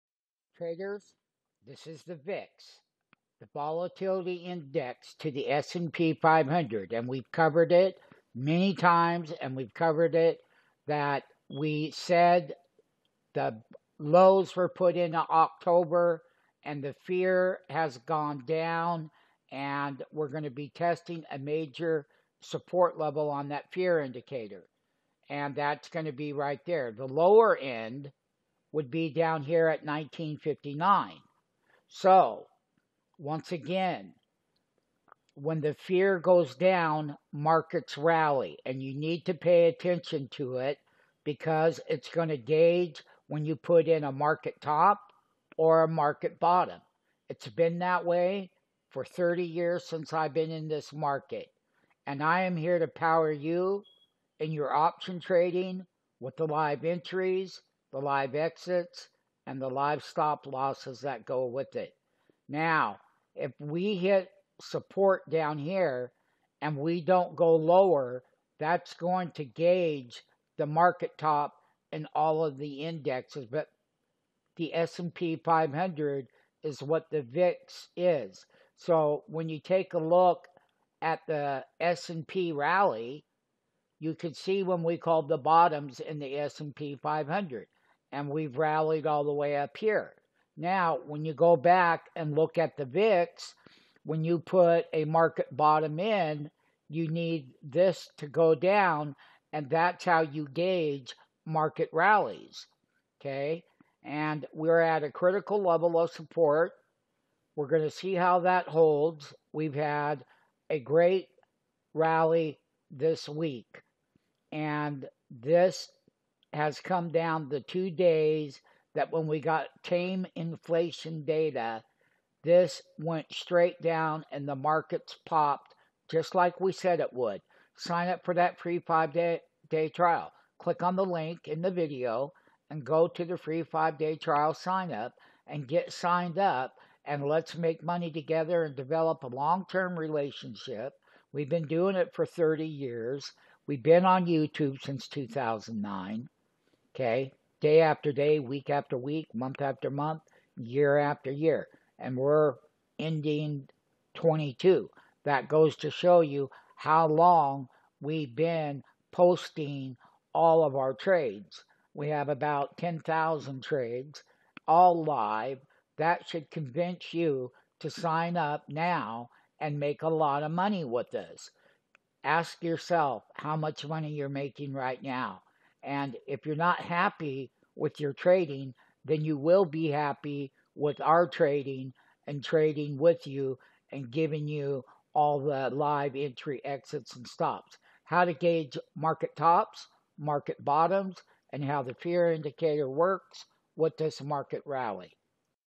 11 12 22 VIX cboe volatility index fear indicator
VIX fear indicator shows lower fear how to gage markets